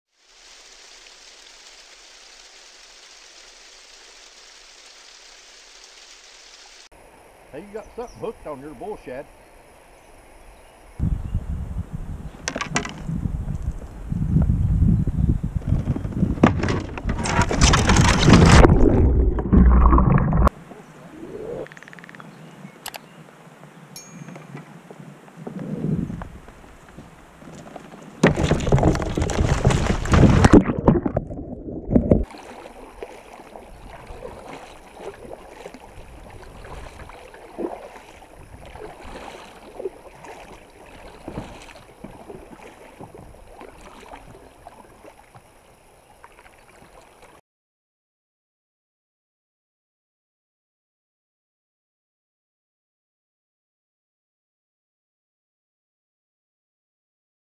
Angler Flips Kayak In River
Plastic seat buckles break on 2022 Jackson Coosa HD kayak. This causes the angler to suddenly shift his weight to one side throwing off the center of balance which results in the kayak fliupping over.